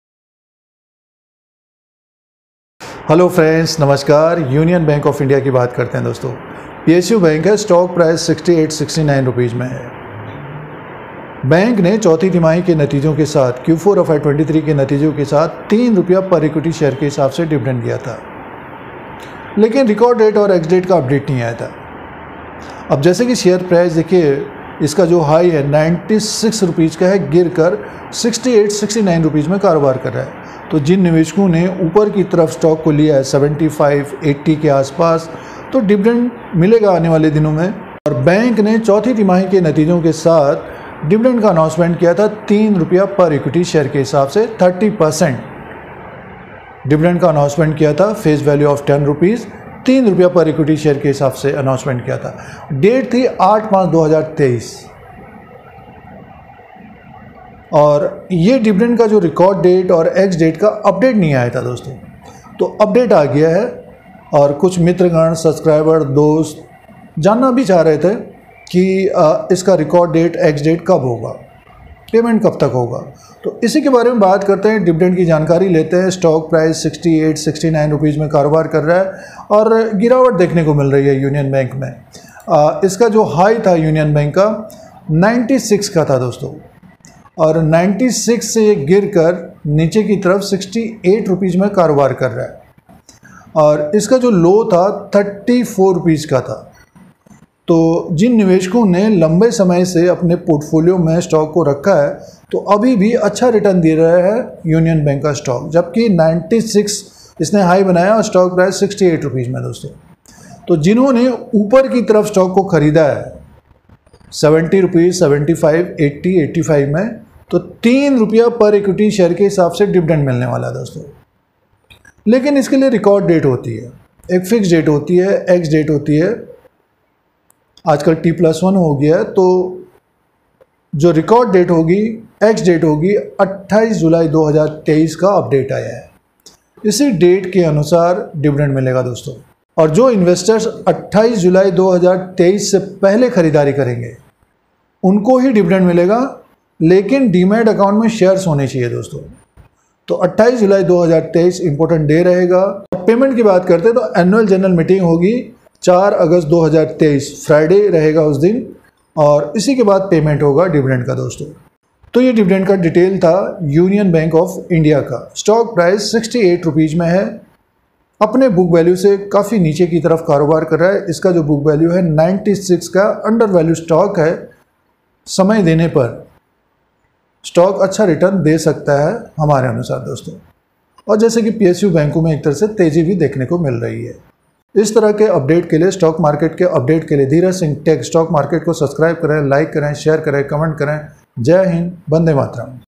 Union Bank Of India Dividend 2023 | Union Bank ₹3 Dividend - Ex Date Record Date - Payout Date कब है
Union Bank of India Dividend 2023 Union Bank ₹3 Dividend - Ex date -Record Date - Payout Date कब है Union Bank Of India Share News Dhiraj Singh tech Stock Market

Covered Topic -

Watch YouTube channel DhirajSingh tech Stock Market